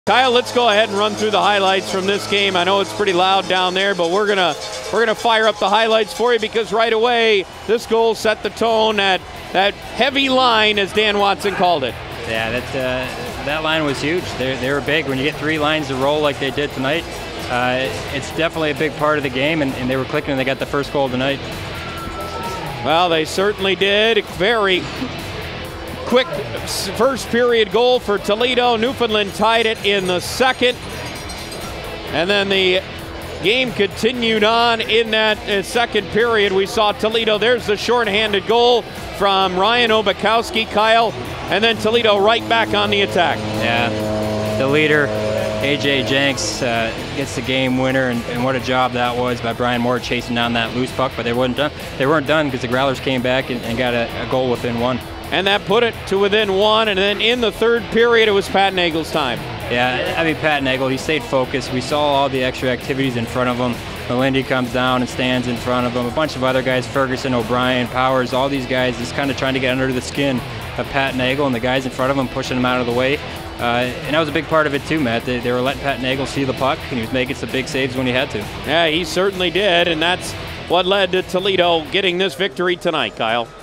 Kelly Cup Finals Game 5 Highlights (6/1/2019)
Toledo Walleye 3 vs Newfoundland Growlers 2